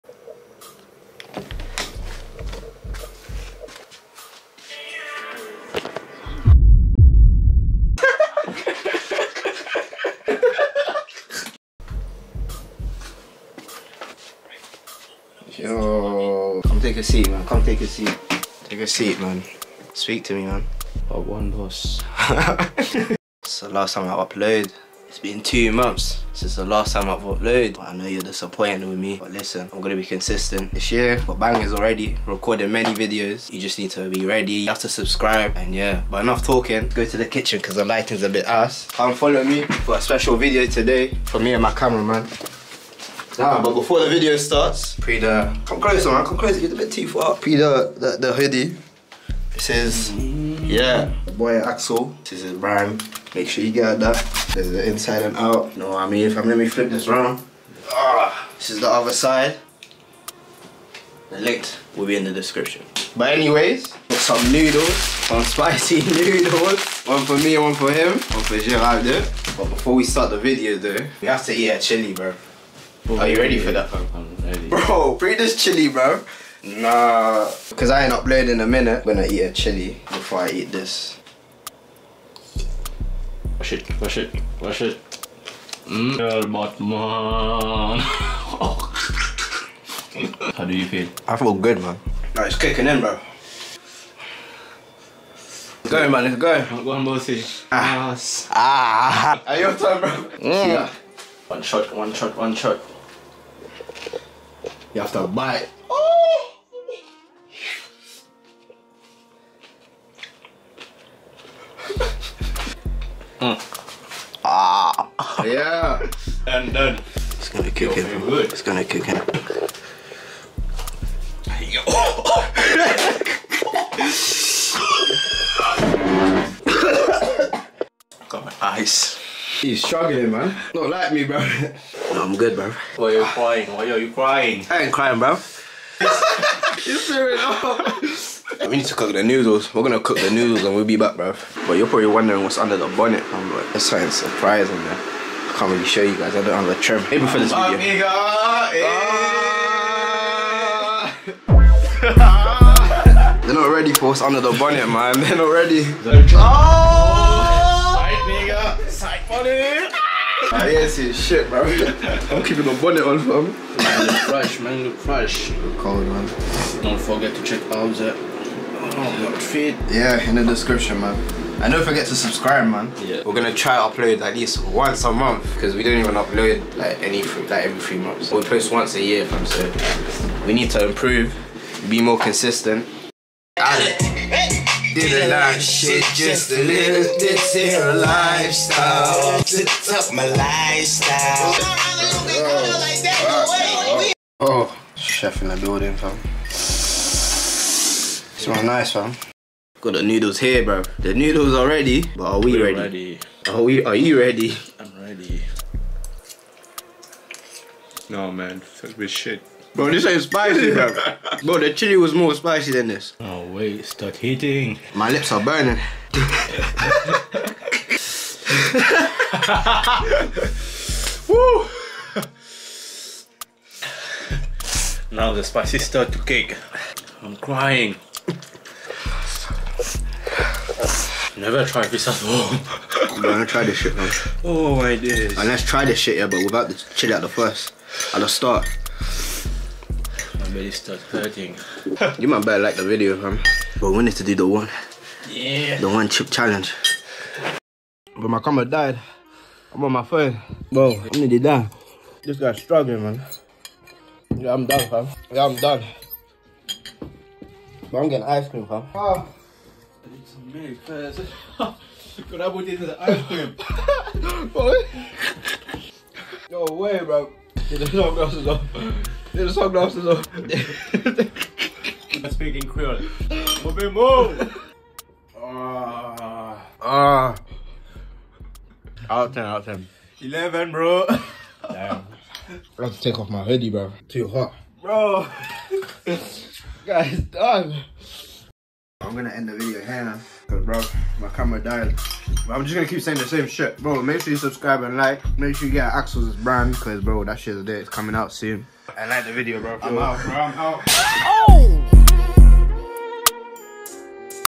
EXTREME SPICY RAMEN NOODLE CHALLENGE !!
we wasn't expecting the noodles to be that spicy, thought it was all a joke.

In the comment section, write down some video ideas I should do     :)